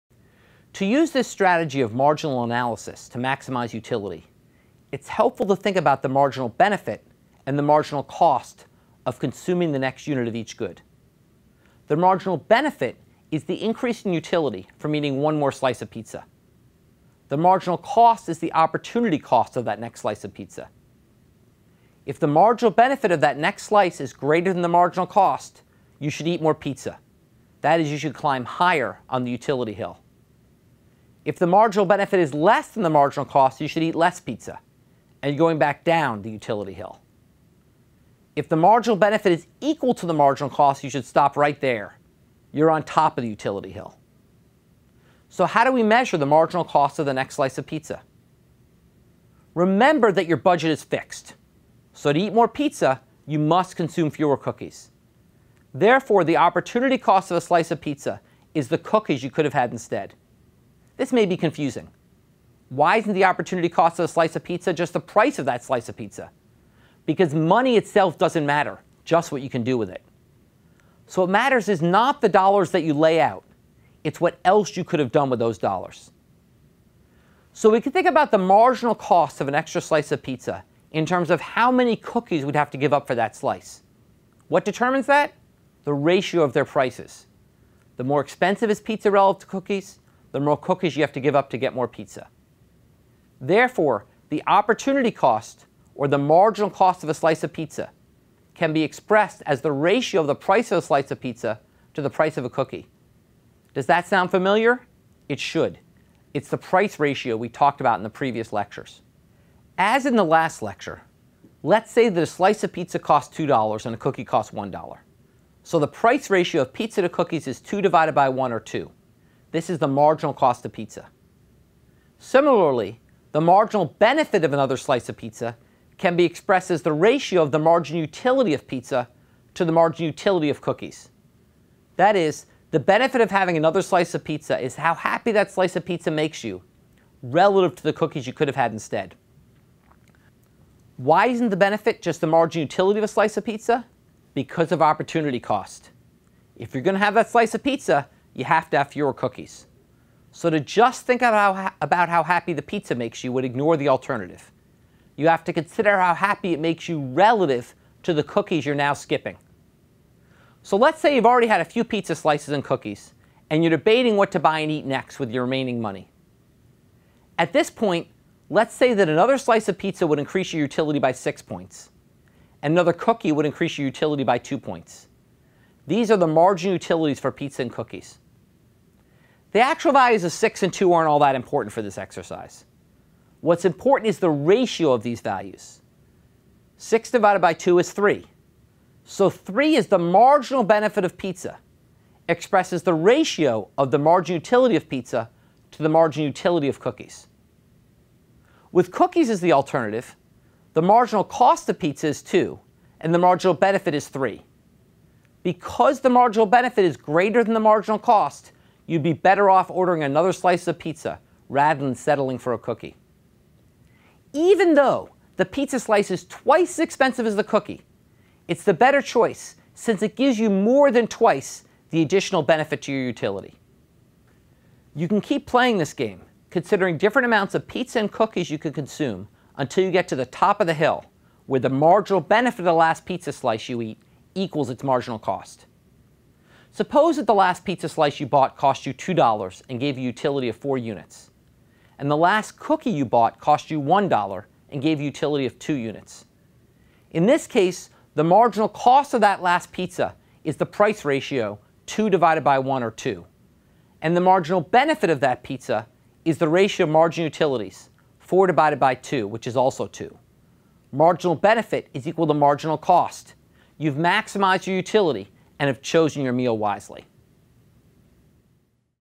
1.7 Utility Maximization Part II
How does a consumer decide how much of something to buy? And what does this have to do with climbing a hill?